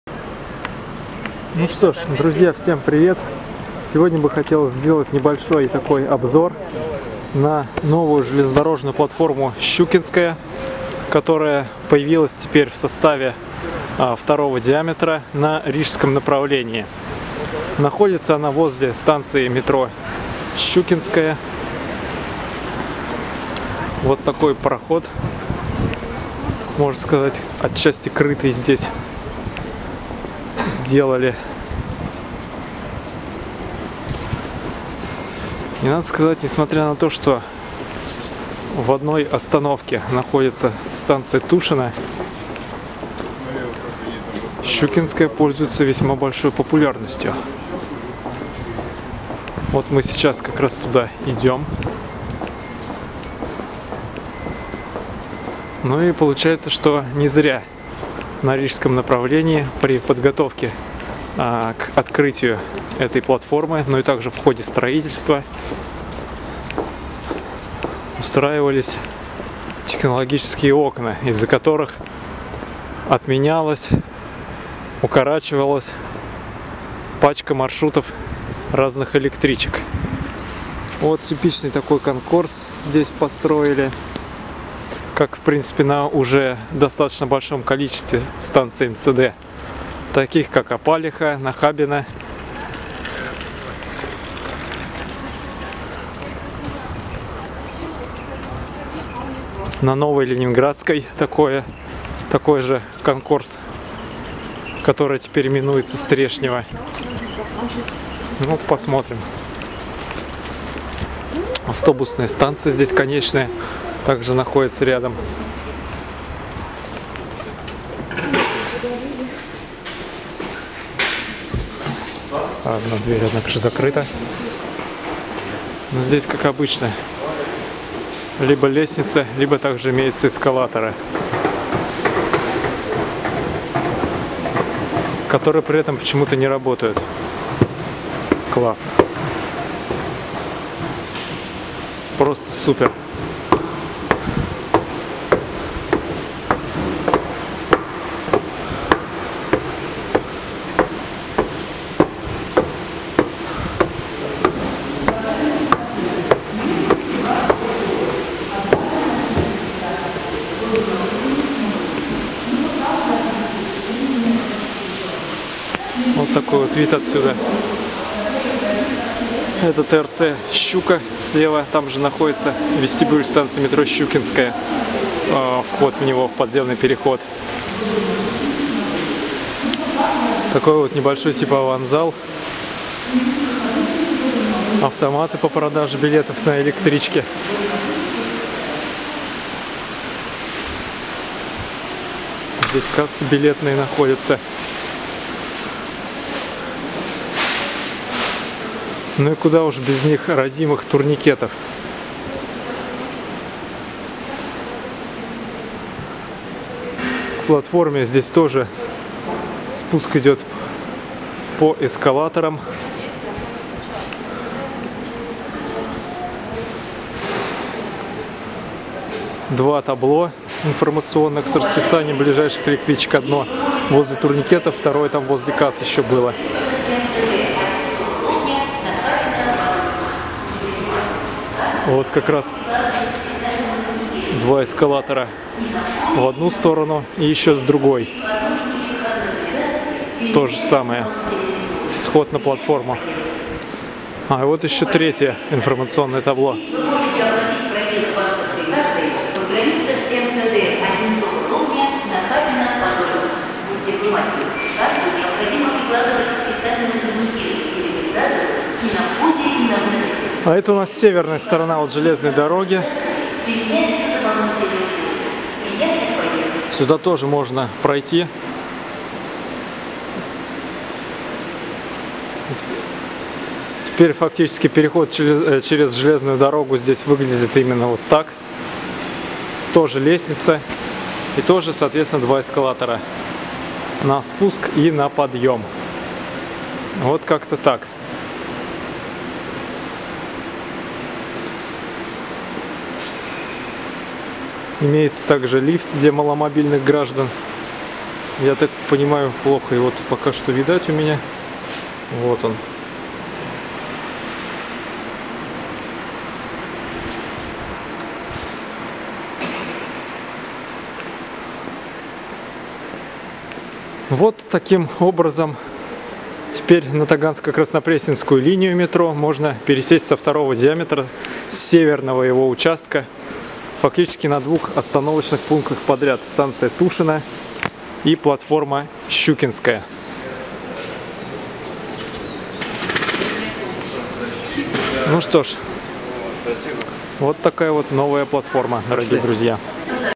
Обзор на новую платформу "Щукинская" МЦД-2 в Москве на Рижском направлении
Небольшой обзор на новую платформу "Щукинская" МЦД-2 в Москве на Рижском направлении и ее окрестности.